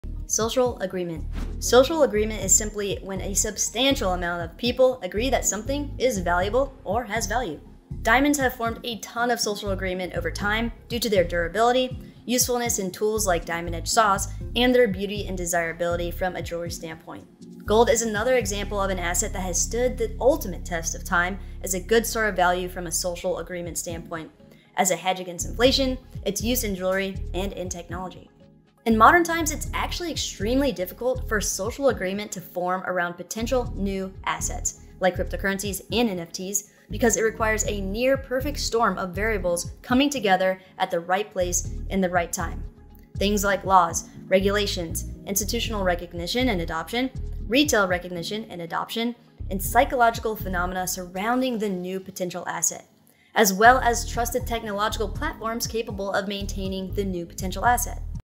Social Agreement of Assets (In 1 Minute!) - Beginners’ Guide
This is a short video explaining what "social agreement" means with regard to assets and their valuations.